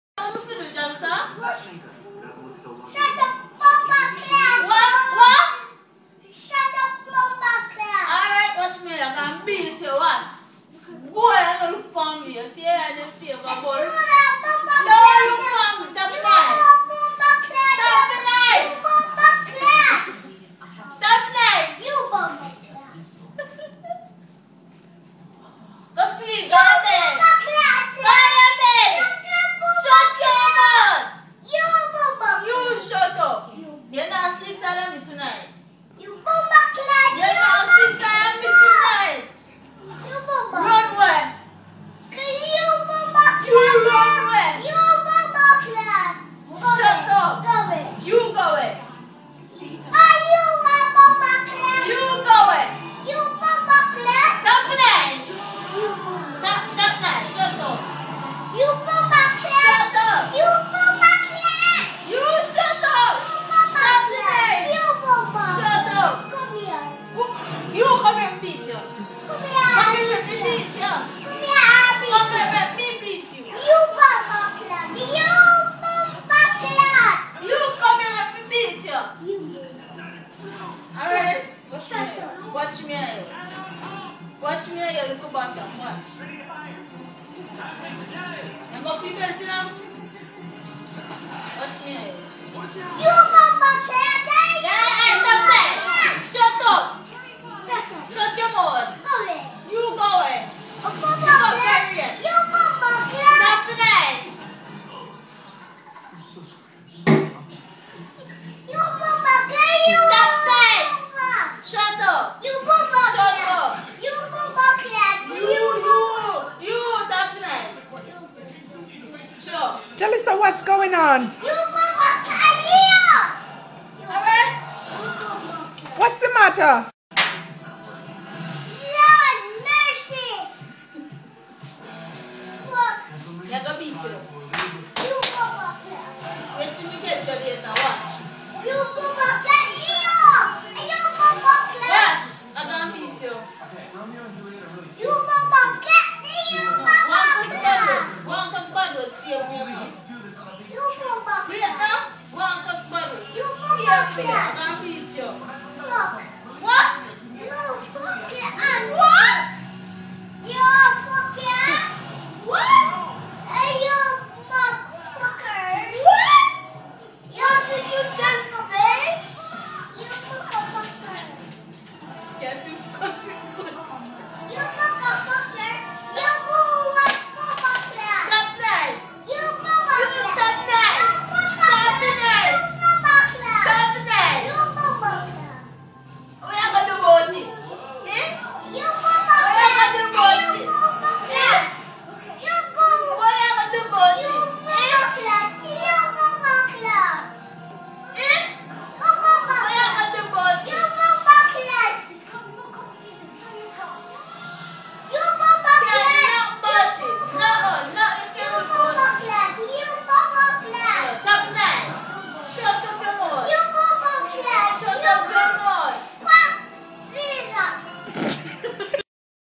Little Jamaican girl cussing
Kids are like sponges, they absorb everything that is said infront of them and then repeat it. Parents are to be blamed for her behavior